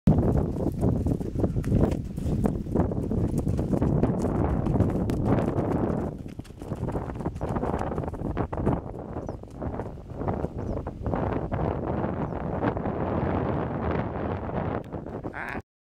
Joe and Baby Cakes shows how she moves and her versatility
She can gait,fast or slow, lope, gallop.  She can do it all.  Plus she's kid broke.